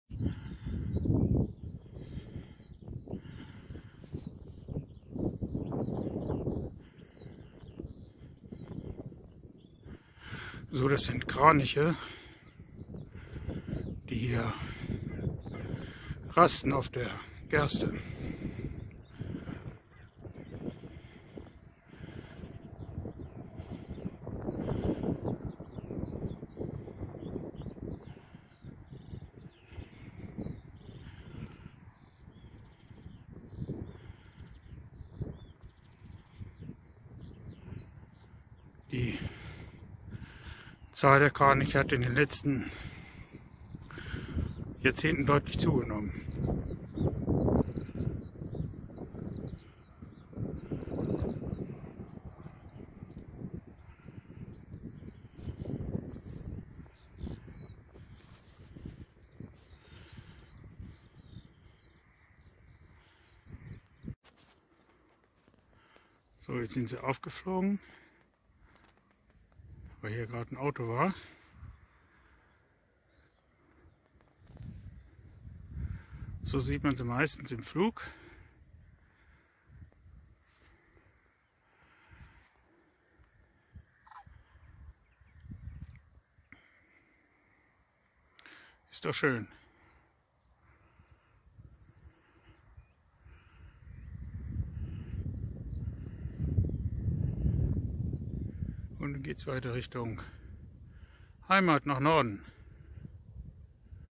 Kraniche
Kraniche rasten auf der Gerste.